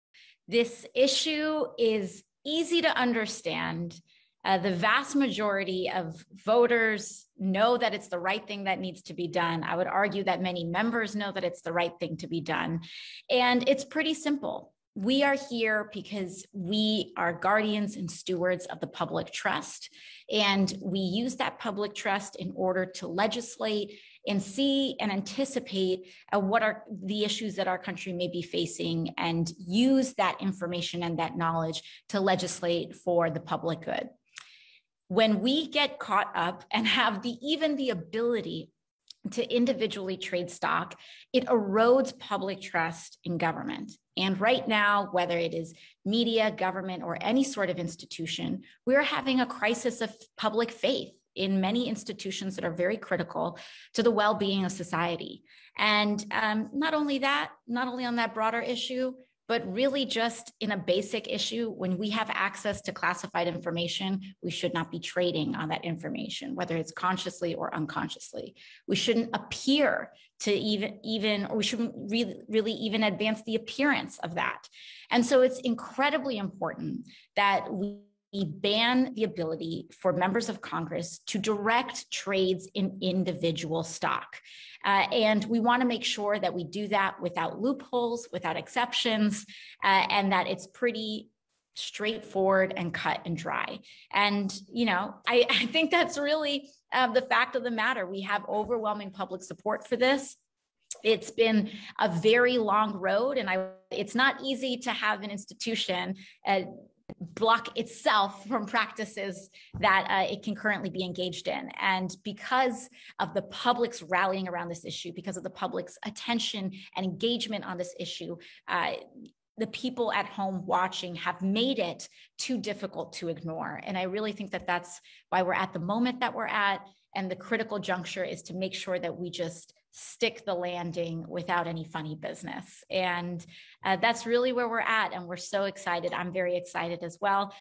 'Erodes The Public Trust': Ocasio-Cortez Calls On Congress To Ban Lawmakers From Trading Stocks
Rep. Alexandria Ocasio-Cortez (D-NY) told supporters that lawmakers need to ban members of Congress and their senior staffers from trading individual stocks, as momentum grows in both chambers to develop legislation on the issue.